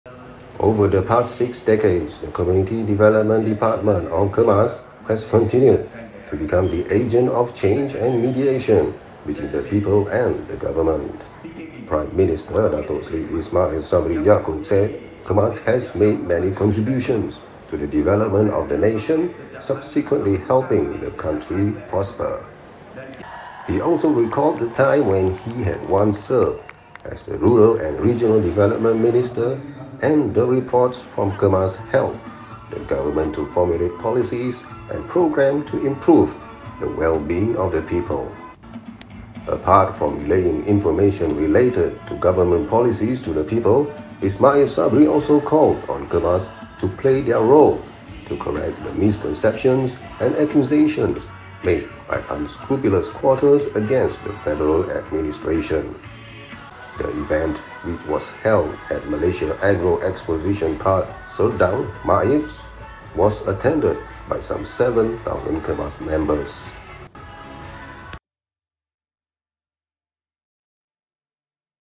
Kemas urged to help correct misconceptions against federal administration
Over the past six decades, the Community Development Department (Kemas) has continued to become the agent of change and mediation between the people and the government.

Prime Minister Datuk Seri Ismail Sabri Yaakob said Kemas has made many contributions to the development of the nation, subsequently helping the country prosper.

He also recalled the time when he had once served as the rural and regional development minister, and the reports from Kemas helped the government to formulate policies and programmes to improve the well-being of the people.

Apart from relaying information related to government policies to the people, Ismail Sabri also called on Kemas to play their role to correct the misconceptions and accusations made by unscrupulous quarters against the federal administration.

The event, which was held at the Malaysia Agro Exposition Park Serdang (MAEPS), was attended by some 7,000 Kemas members.